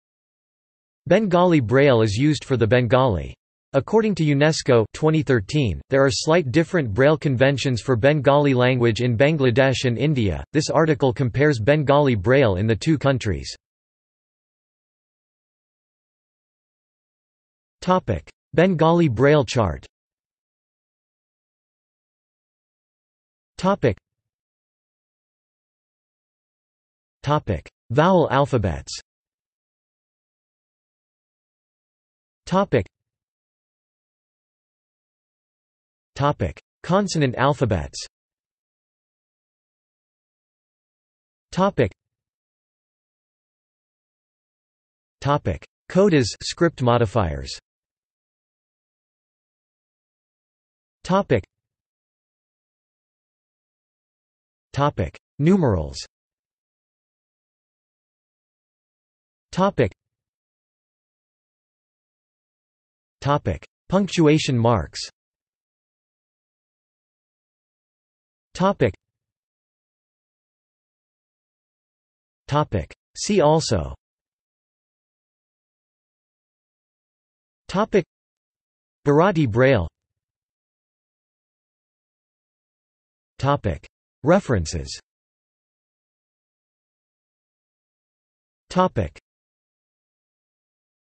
Bengali Braille | Wikipedia audio article
This is an audio version of the Wikipedia Article:
Bengali Braille

SUMMARY
Bengali Braille is used for the Bengali. According to UNESCO (2013), there are slight different braille conventions for Bengali language in Bangladesh and India, this article compares Bengali Braille in the two countries.